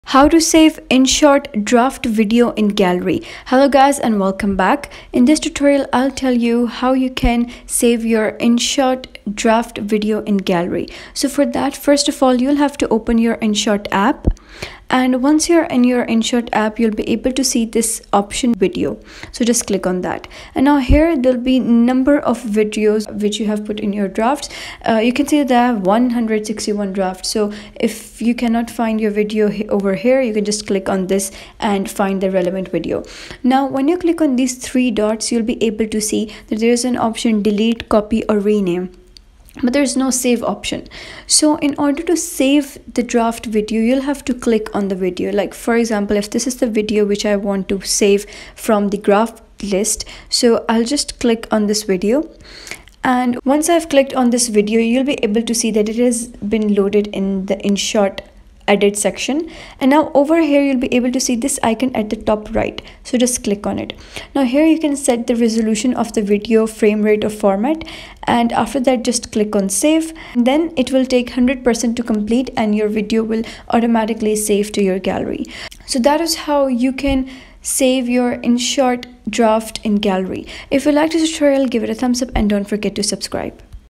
How To Save InShot Draft Video in Gallery (2025)
In this video I'll show you how to safe inshot draft video in gallery. The method is very simple and clearly described in the video. Follow all of the steps & save inshot video draft to gallery